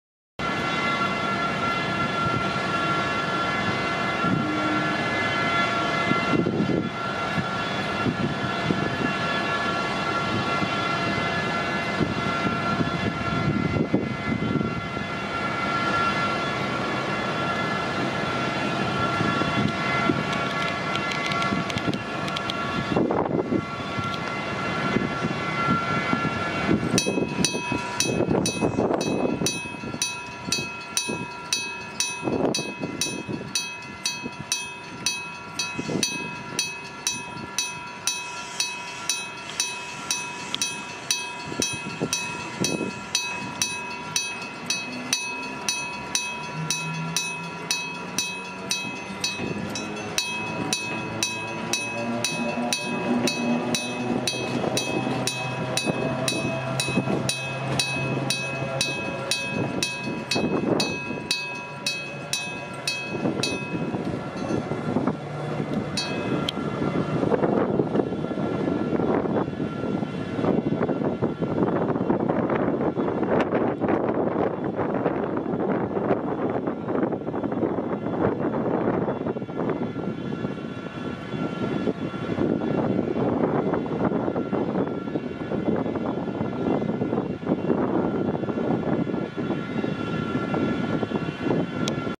三井化学専用鉄道宮浦操車場 Mitsui Chemicals Industrial Railway Freight Train
1917（大正6）年製12号機20t B形電気機関車がコキ200形を牽引する工場行き列車が東泉町2号踏切を通過する。
Miike Railway heritage electric locomotive hauling a container train will depart from Miyaura station, Fukuoka Prefecture. This locomotive was manufactured in 1917.